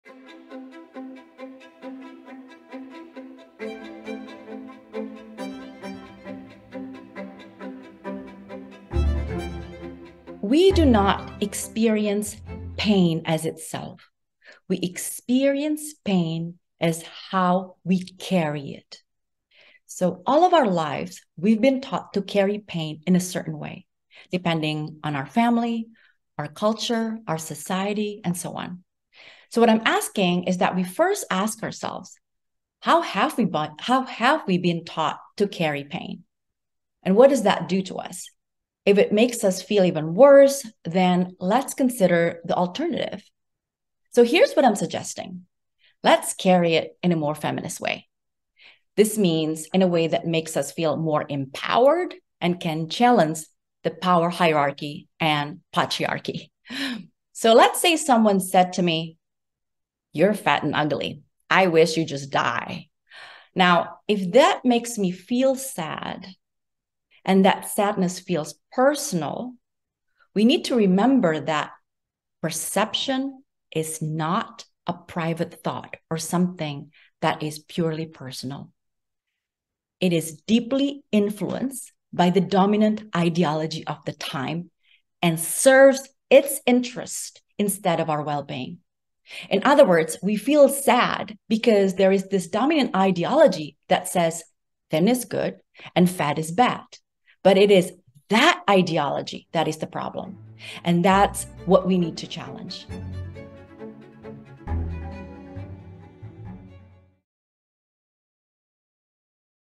Scarred: A Feminist Journey Through Pain - L. Ayu Saraswati
What can we ask of pain? L. Ayu Saraswati provides thought-provoking theories and life-transforming ways to understand pain, specifically in relation to feminism.